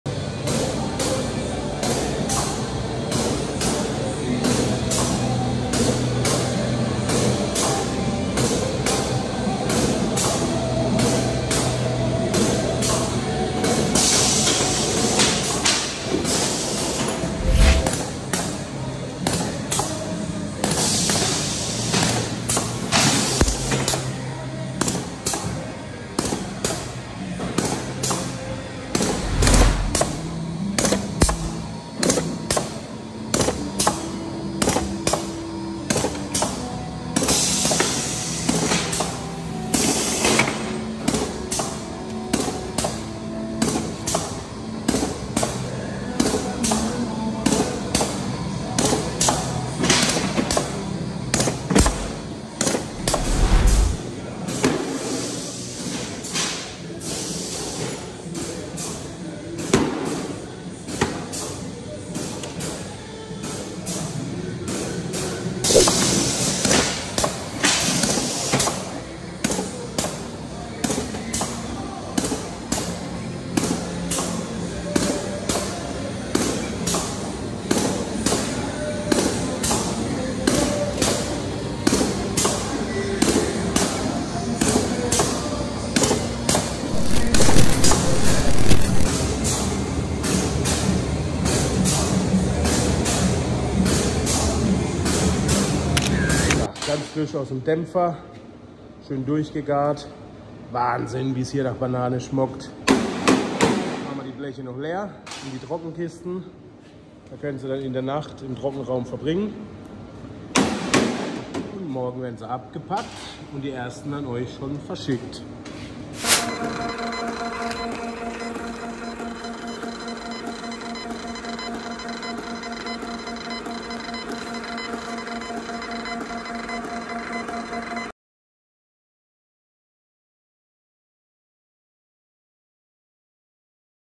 Nautika Baits - Inside Authentic Banana - Hinter den Kulissen
Wir geben erneut Einblick in unsere Produktionsstätte, wo Eure Lieblingsköder produziert werden 😍

Wir haben bereits gesehen, dass einige User von Euch schon entdeckt haben, dass der Köder bereits vor dem offiziellen Release am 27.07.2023 im Shop entdeckt wurde. Wir sind tatsächlich überwältigt von Eurem Zuspruch und dem Vertrauen, welches Ihr in unsere Range setzt. Dafür an dieser Stelle ein herzliche Dankeschön.

Gebt Gas Leute und sichert Euch Eure Boilies, denn wir rollen diesen Boilie Klassiker nur einmal dieses Jahr ab. Wenn die Regale leer sind, dann kommt dieser Boilie frühestens erst wieder im nächsten Jahr (eventuell auch andere Special Editions - wir wissen es noch nicht)

Wir wünschen Euch allen viel Erfolg mit unserem Authentic Banana und hoffen darauf, dass Ihr einige Sternstunden am Wasser verbringen könnt. Schickt uns Eure Erfahrungsberichte und Eure Fangfotos per social Media DMs. Wir freuen uns mega darüber.

Tight lines